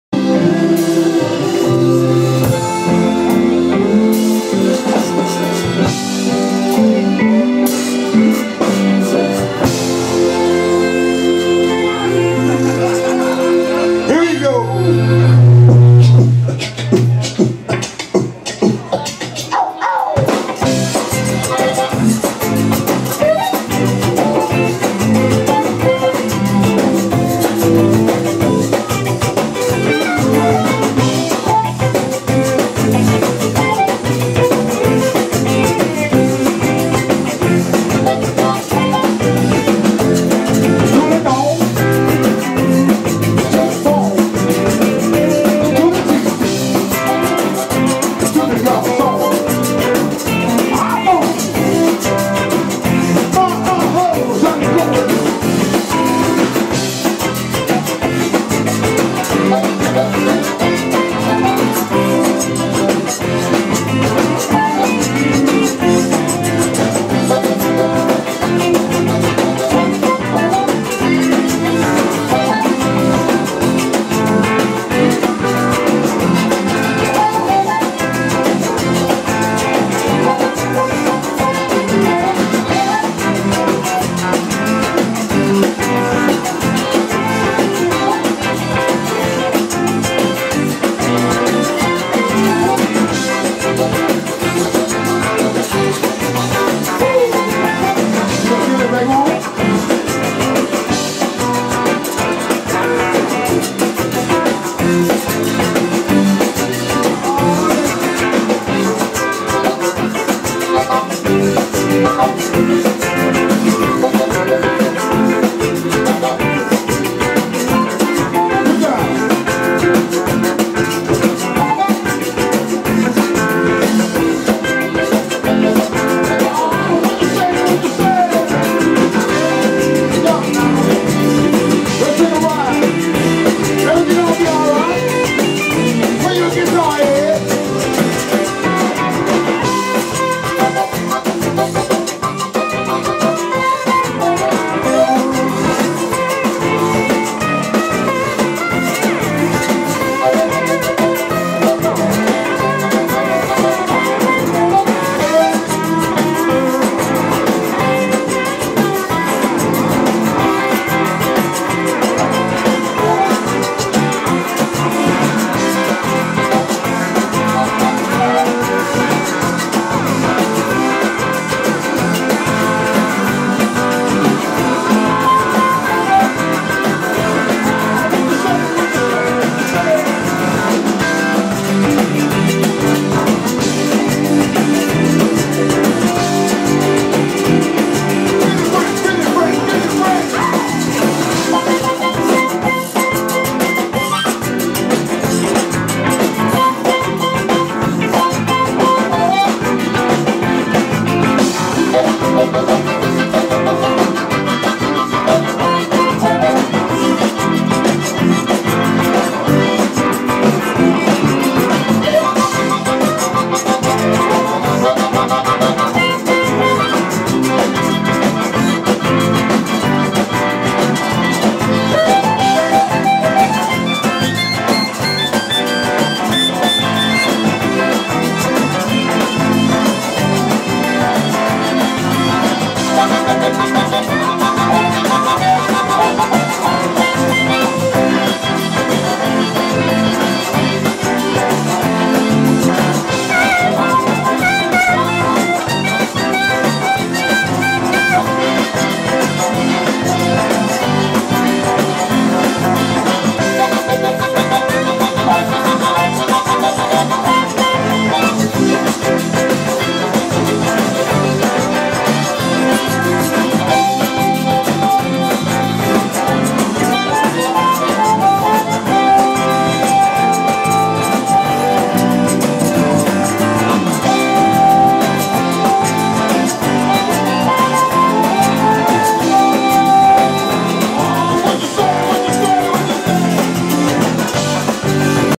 Billy Branch with CJ Chenier and Lil Ed
The son of the "King of Zydeco," C.J. Chenier  jams with Chicago Blues Legends Billy Branch & Lil' Ed at Rosa's Lounge.